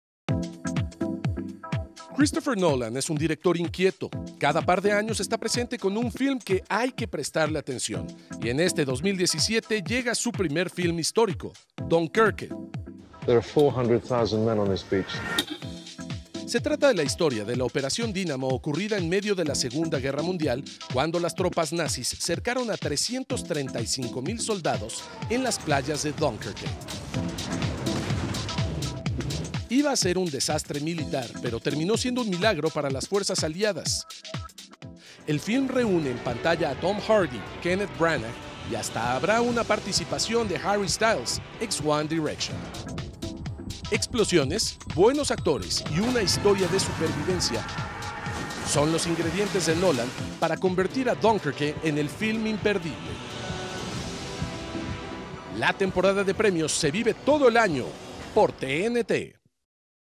TNT News | Dunkerque
Christopher Nolan dejó de lado a los super héroes para dedicarse a los héroes reales. Dunkerque es la historia de cómo las tropas francesas lograron escapar hacia Gran Bretaña antes de que los nazis invadieran el país.
La temporada de premios se vive todo el año en TNT.